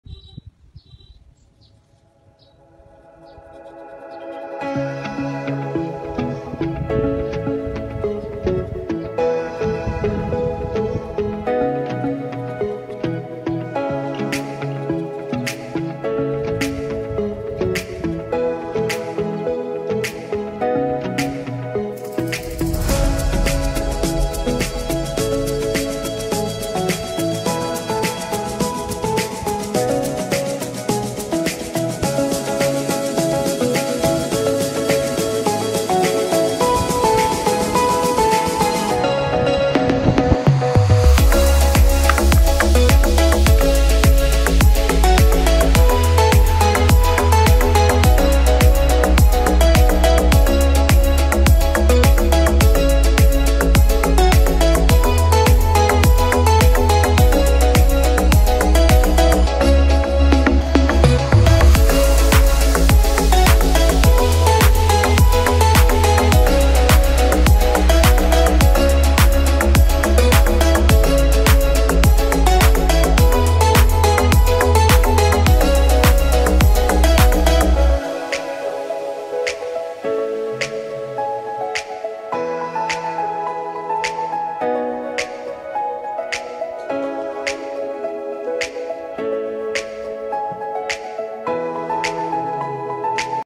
Easy oil color painting with guidance for beginner to choose shades and brushes
natural scene drawing of sunset and trees